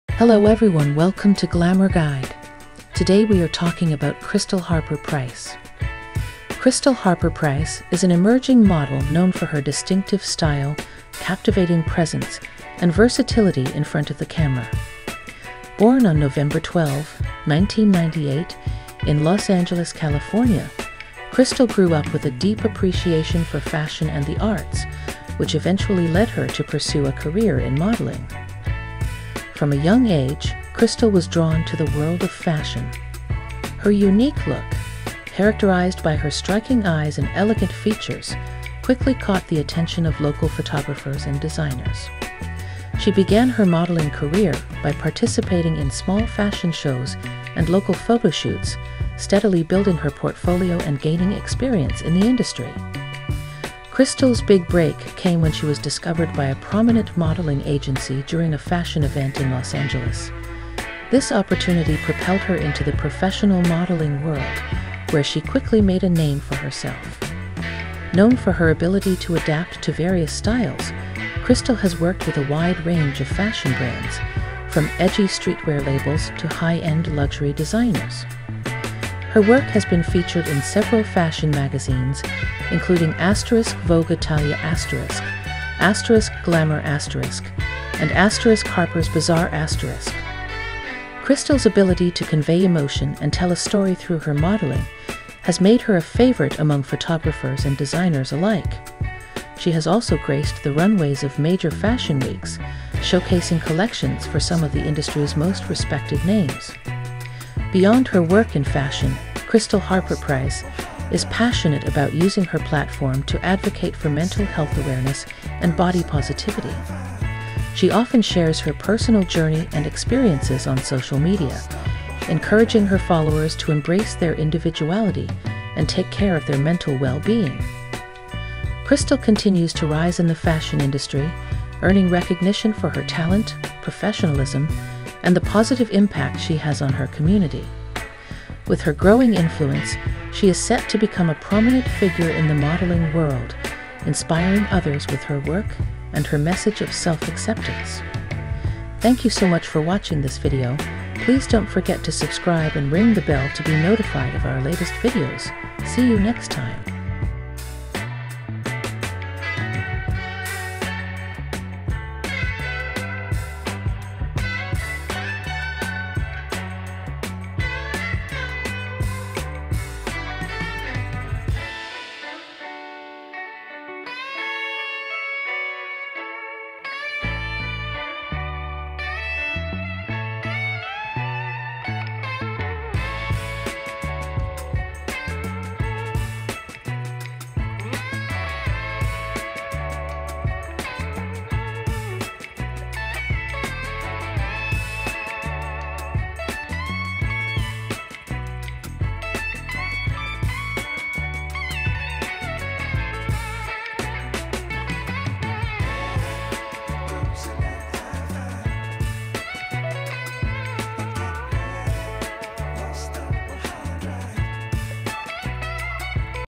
Krystal Harper Preiss: Fashion Model, Social Media Sensation, and More | Biography and Net Worth
Join the fascinating life of Krystal Harper Preiss, the stunning model and social media influencer. From her family to career, and her thriving presence on Instagram and YouTube, get to know Renee's journey. Learn about her fitness secrets, relationships, and estimated net worth in this comprehensive biography!